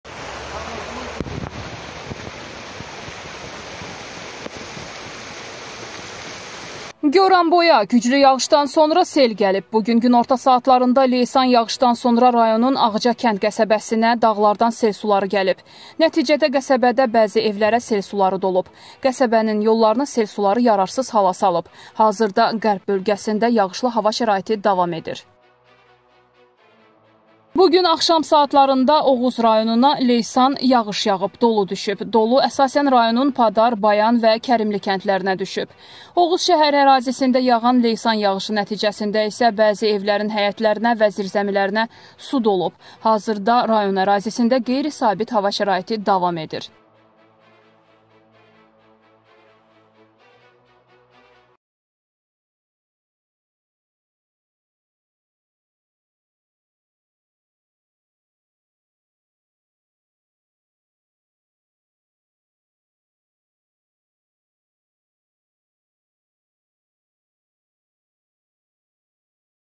Güclü sel bir sıra rayonlarda fəsadlar törədib- GÖRÜNTÜ
Azərbaycanda daha bir lüks avtomobili sel apardı